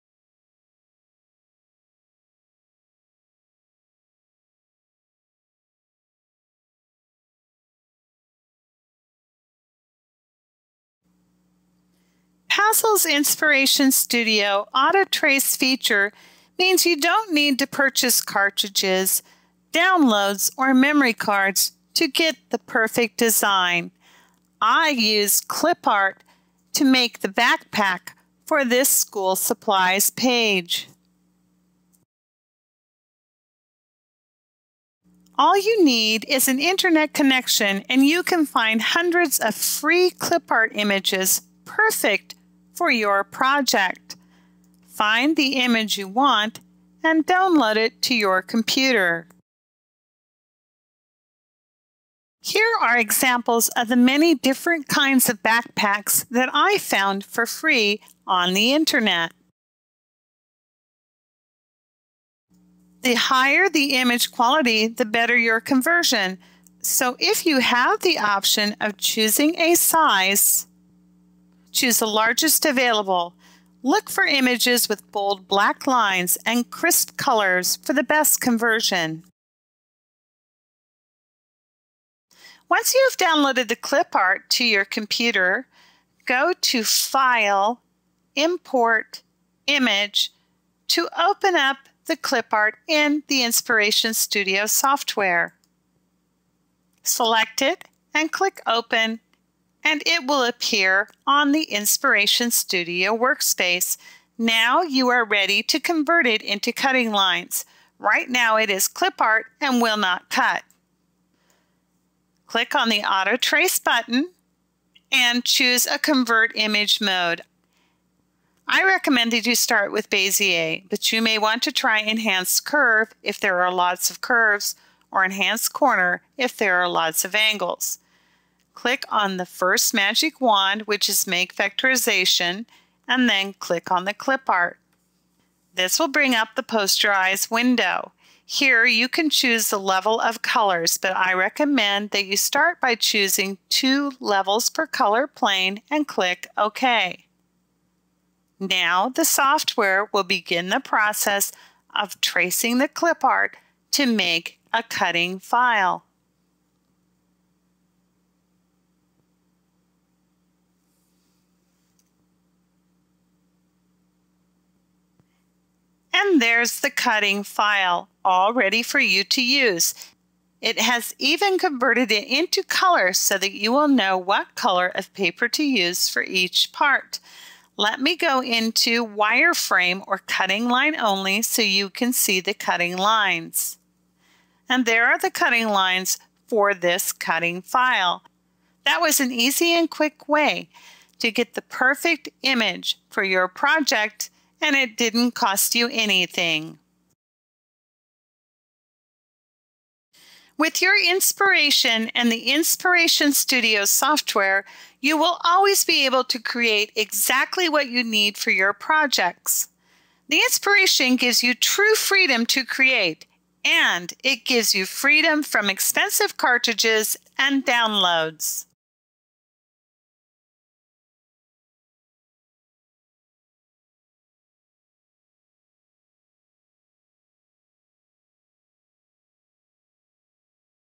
Cutting Files from Clip Art With Auto Trace
Making custom cutting files from your favorite clip art images is easy with the powerful Inspiration Studio Software included with the Pazzles Inspiration!